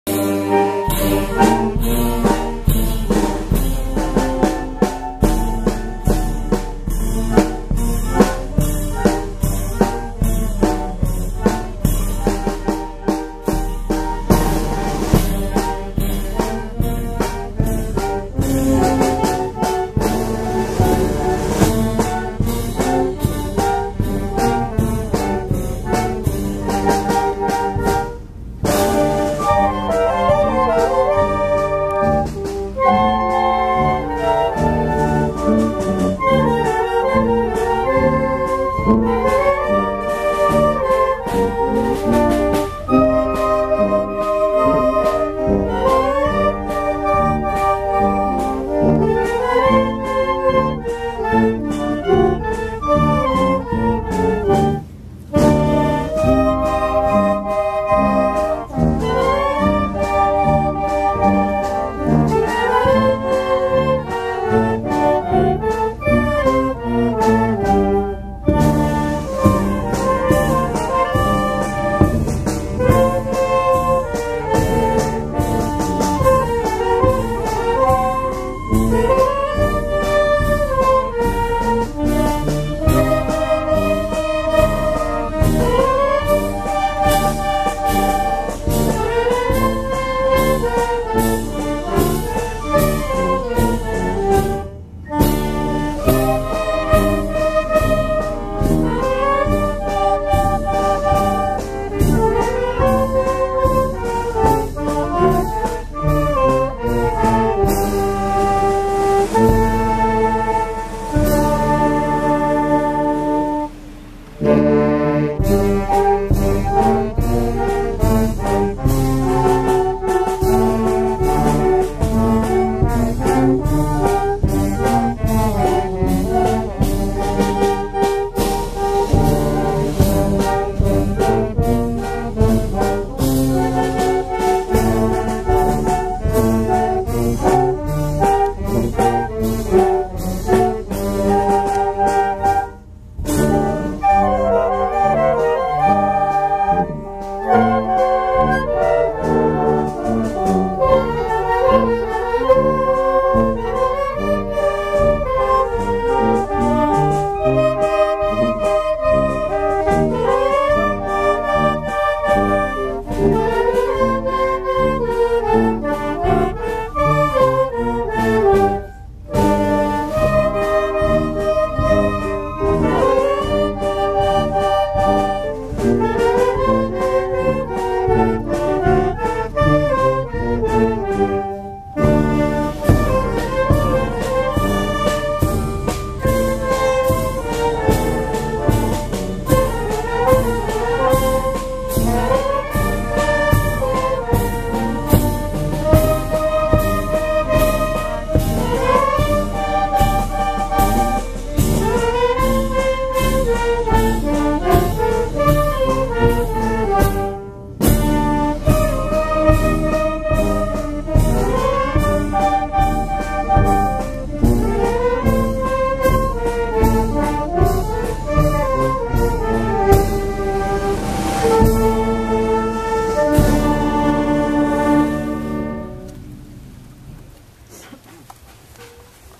Marcha fúnebre 1, a nuestros fieles difuntos.
Banda infantil - juvenil "Unión y Progreso"
de San Miguel Talea de Castro, Oax.
Grabado el 02 de noviembre de 2017
En el panteón municipal.